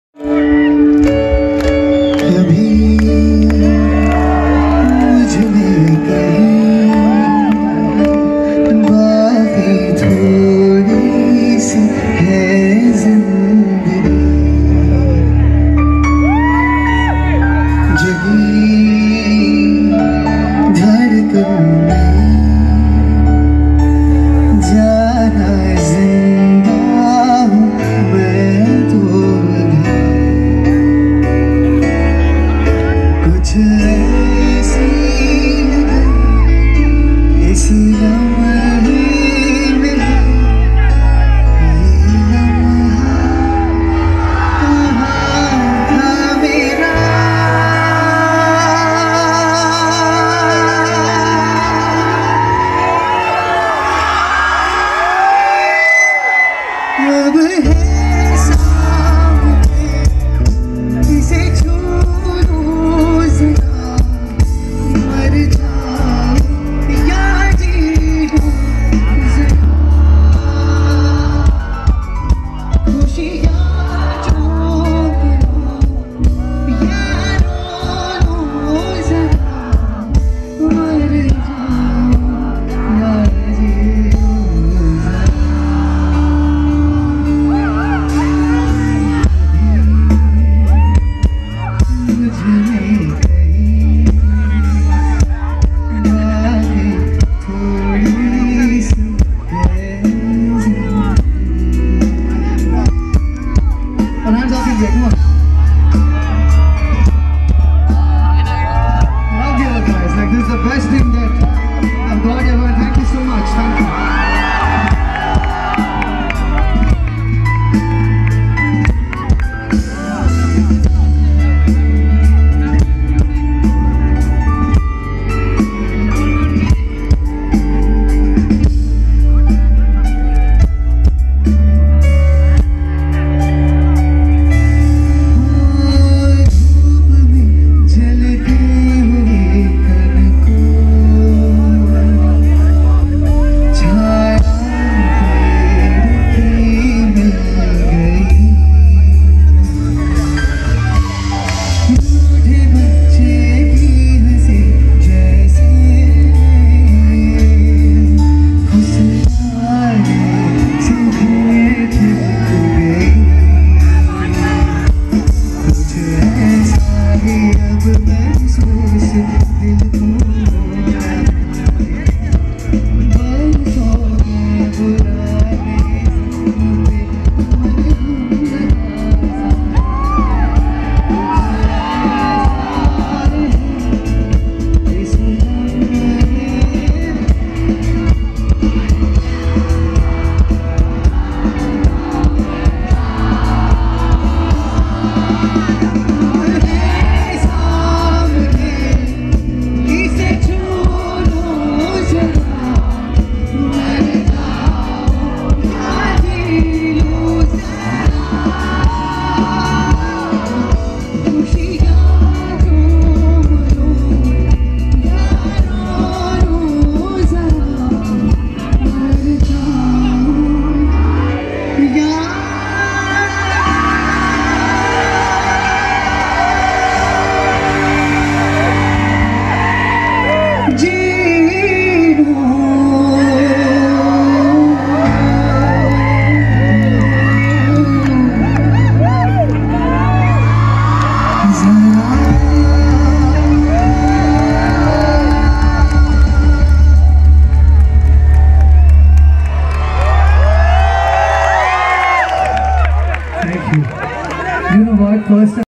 Sidharth Menon performing live @ dyuksha 2020 // NSSCE Palakkad // FHD
beautiful singing of sidharth menon @ dyuksha 2020 nssce palakkd.

dyuksha 2020
sidharth menon live
nssce palakkad